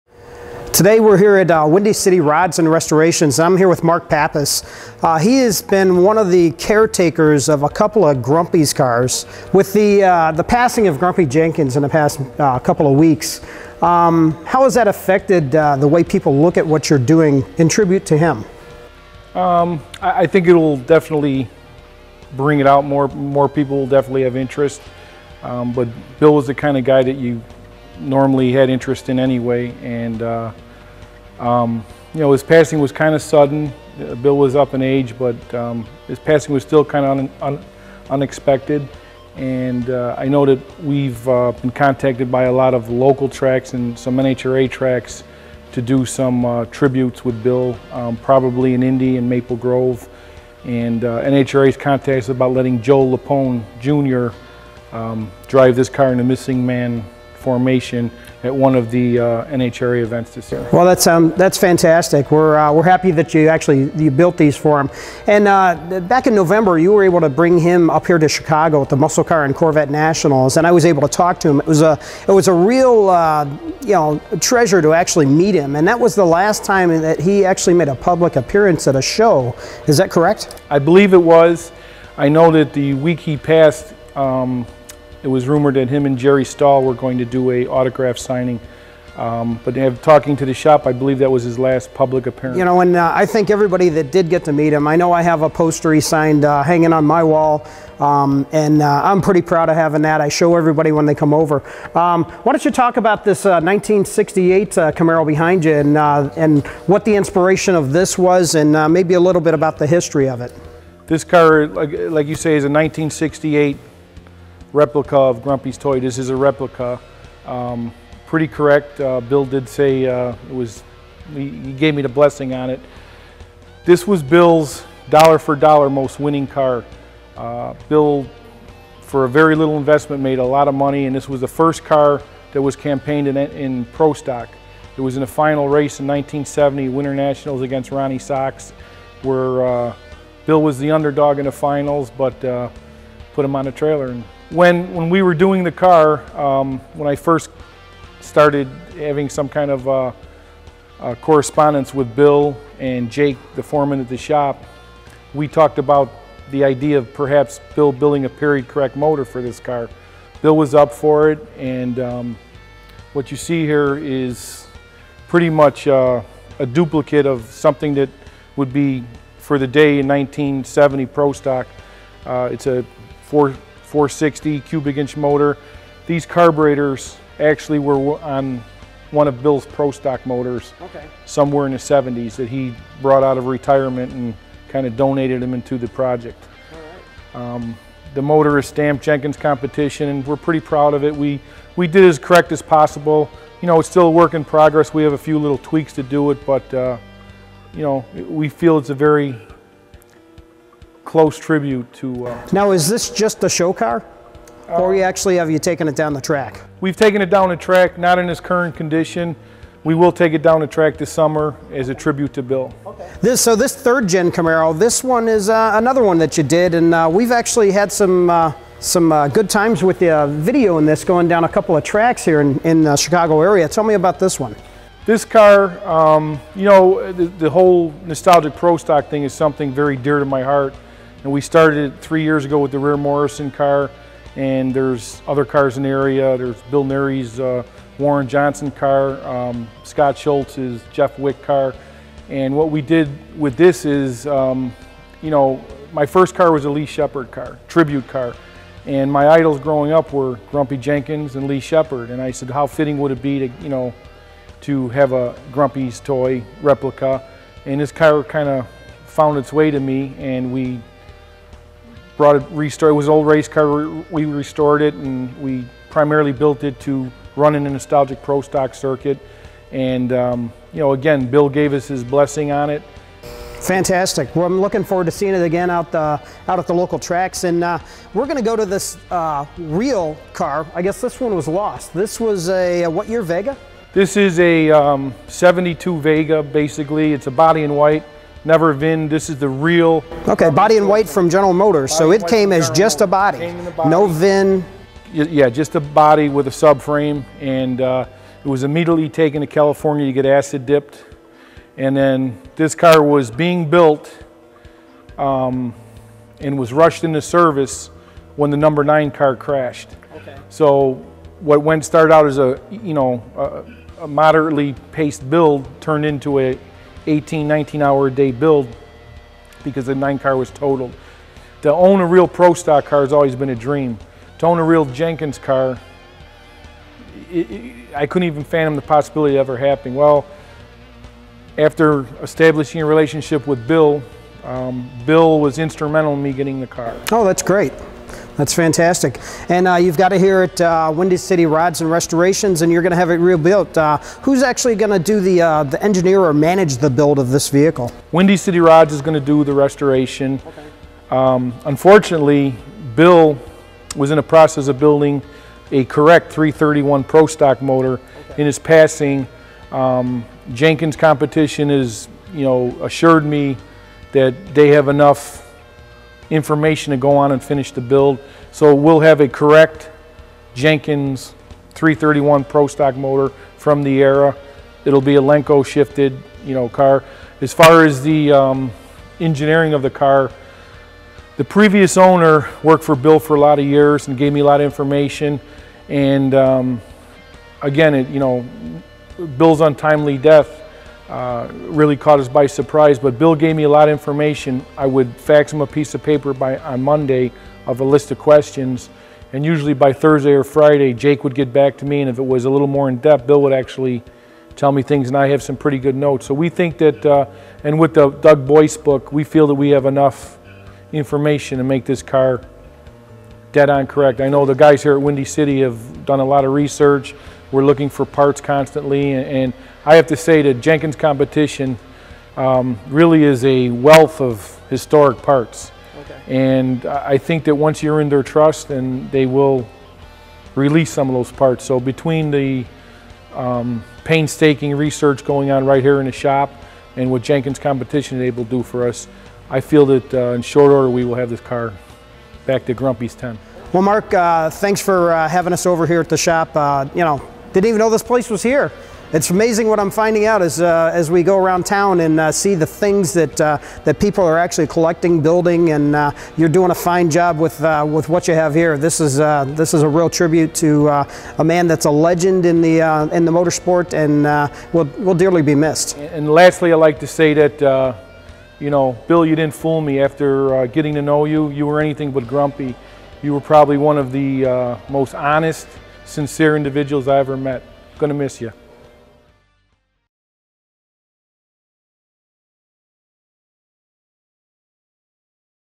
Mark Pappas' Grumpy Jenkins Collection
An interview with Mark Pappas who is not only keeping the memory of Grumpy Jenkins alive but also some of his most memorable race cars